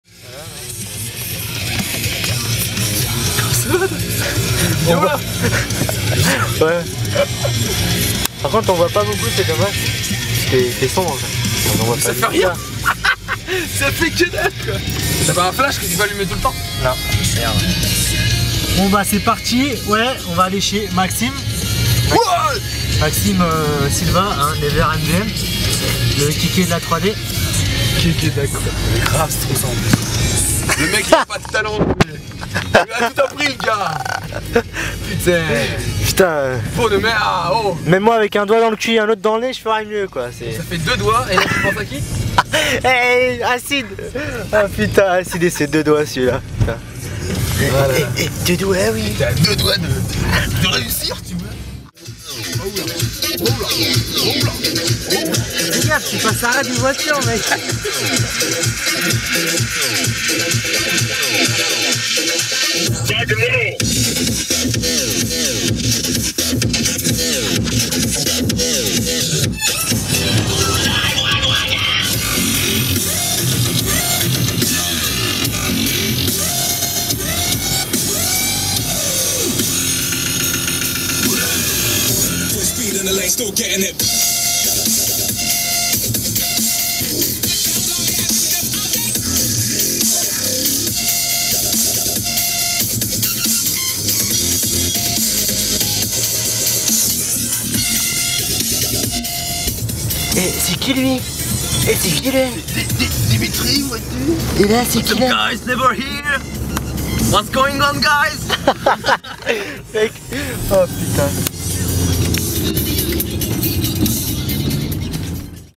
guesh, Dimitri, Never with Dubstep =D
This weekend me and dimitri are coming to Never's house =D